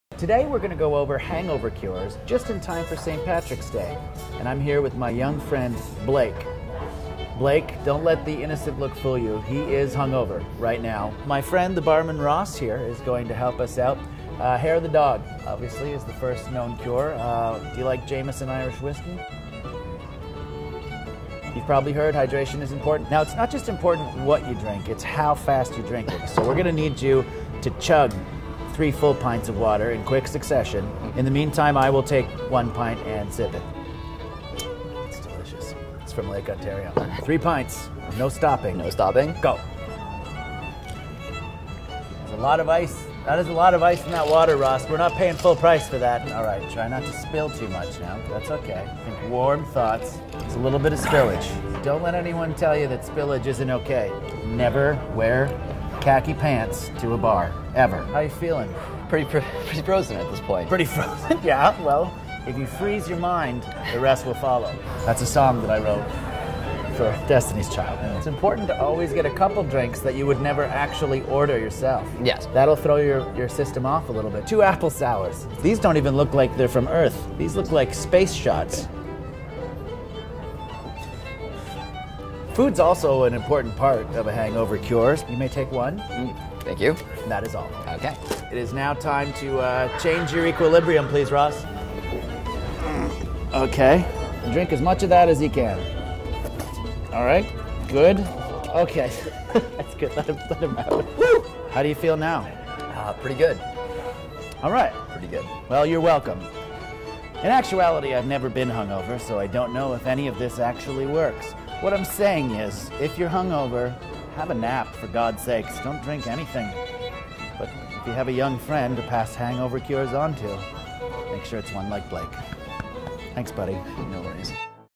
Steve Patterson's "Hangover Cures" | CBC Connects
Steve Patterson, host of CBC Radio's The Debaters, shares his tried and true hangover cures in time for St. Patrick's Day.

About CBC Connect: Go behind the scenes with the CBC's biggest stars.